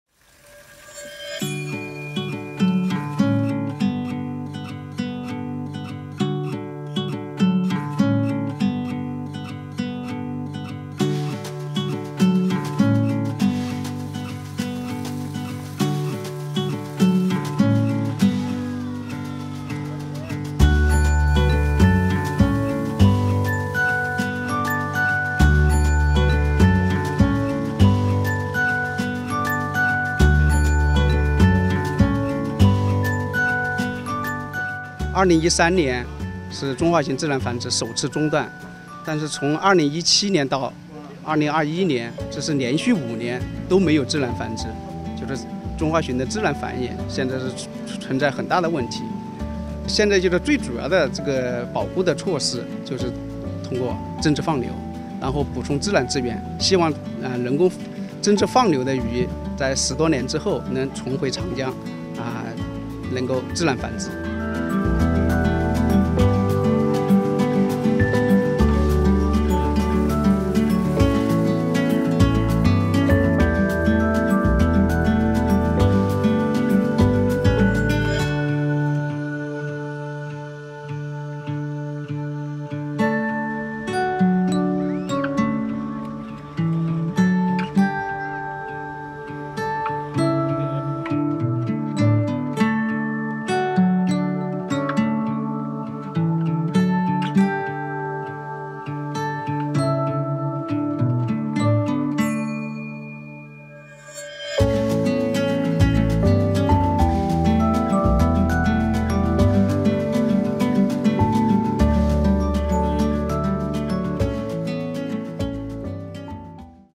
About 50,000 Chinese sturgeon fry, known as ‘aquatic panda’, released into Yangtze River
About 50,000 captive-bred Chinese sturgeon fry have been released into the Yangtze River in China’s Hubei province. Known as “aquatic panda”, the Chinese sturgeon is a national-level protected species in China. It has existed for about 140 million years and is known as a “living fossil” for its high research value. Nevertheless, human activity has caused a sharp decline in the population of Chinese sturgeon in the Yangtze River, to the point where it has become an endangered fish. Chinese researchers have to artificially breed them to ensure the species’ survival.